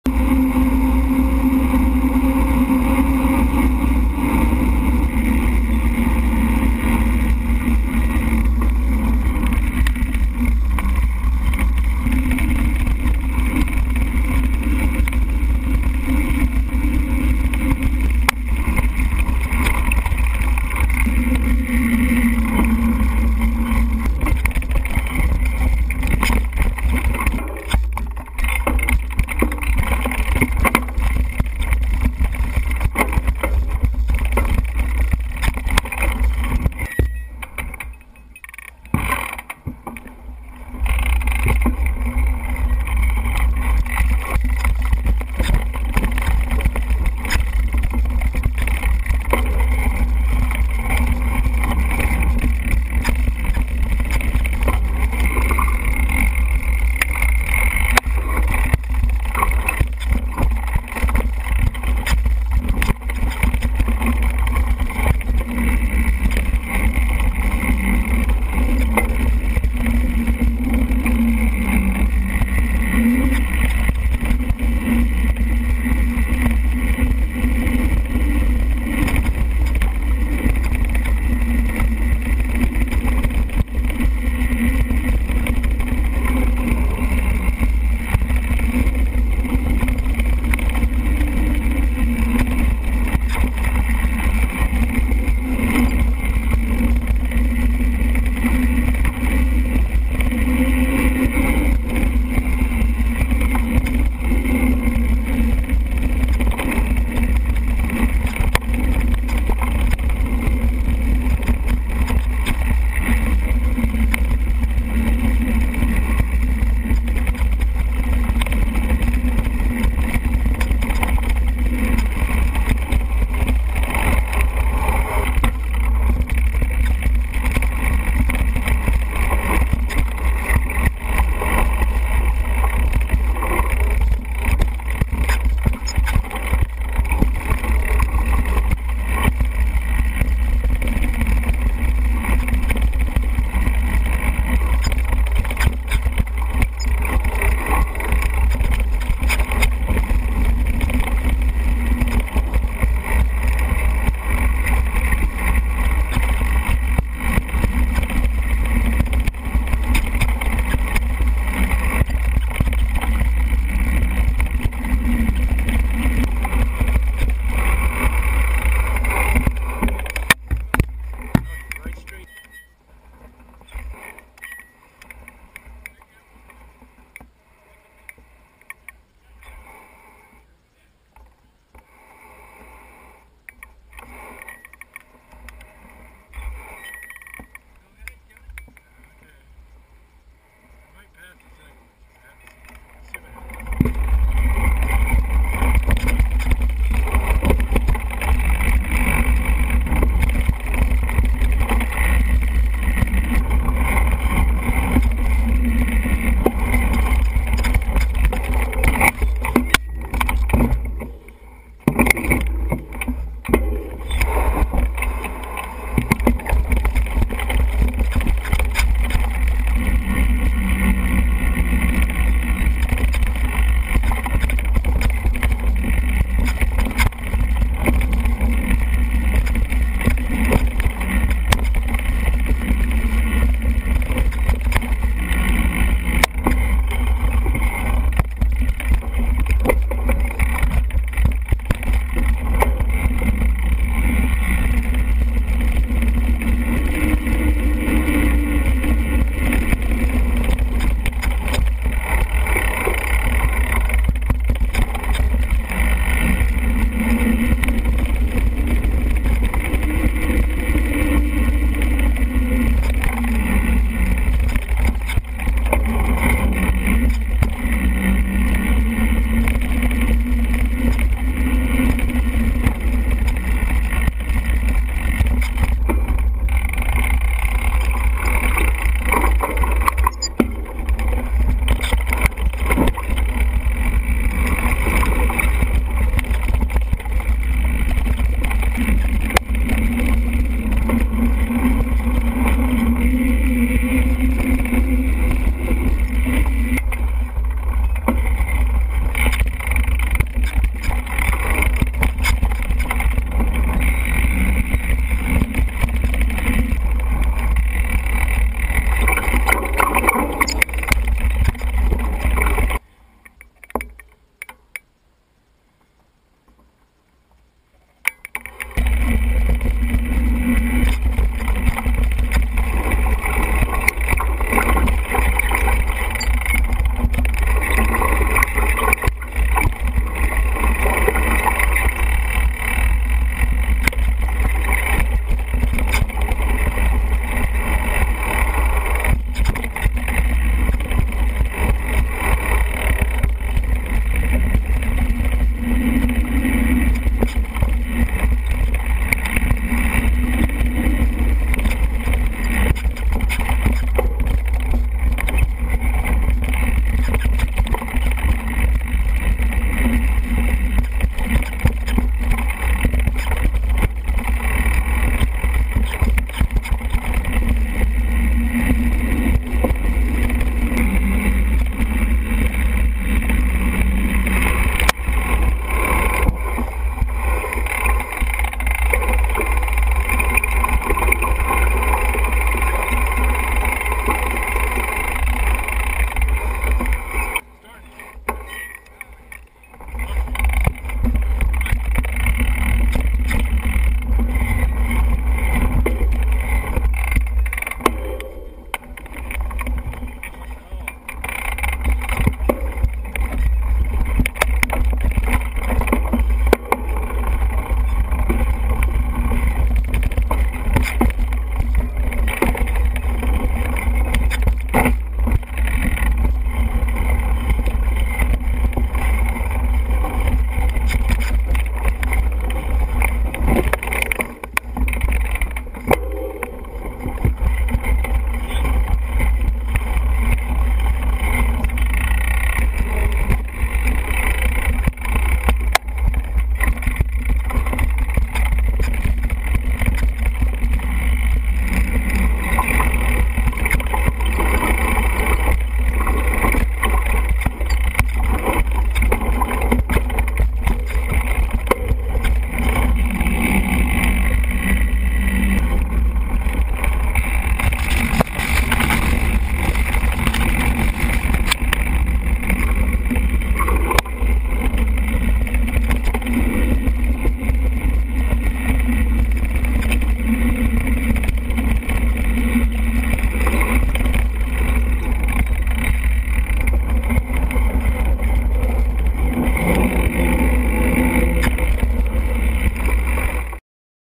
Funner
Highlights of the Funner trail in the Bend area.